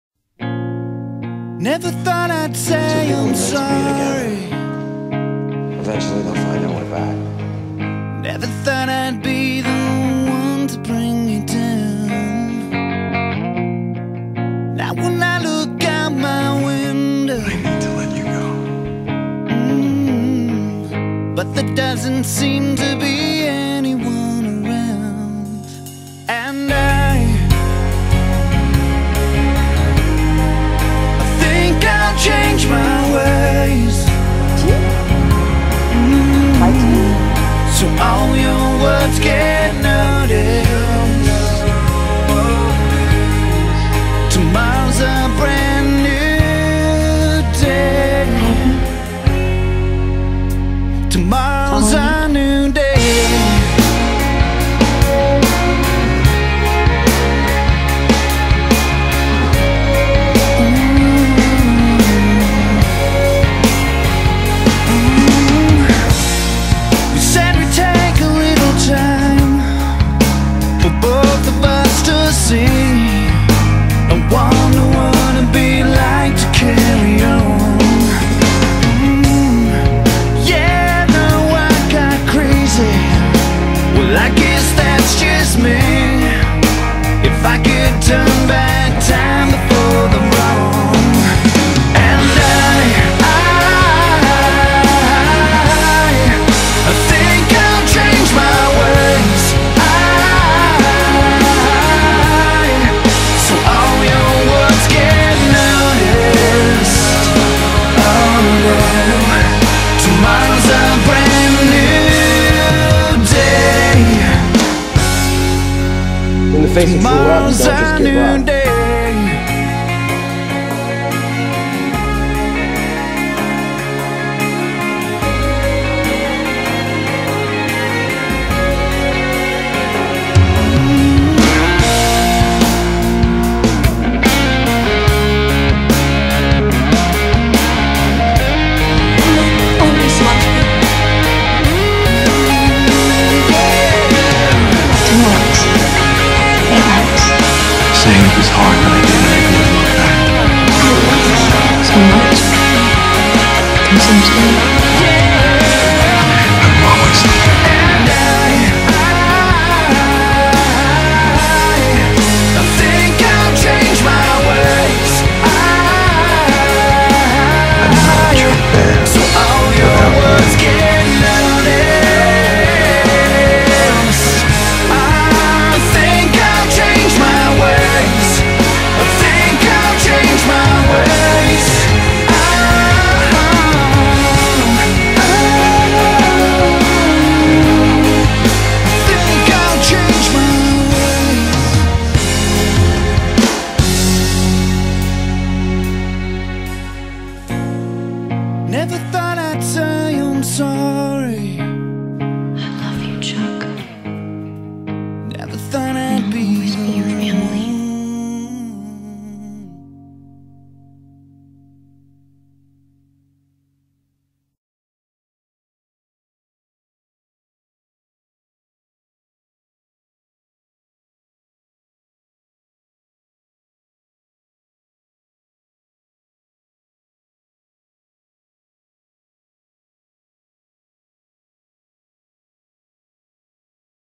chuck and blair; happy limoversary ♥ four years of...
Here's to the future Mr. and Mrs. Bass, and many more happy memories!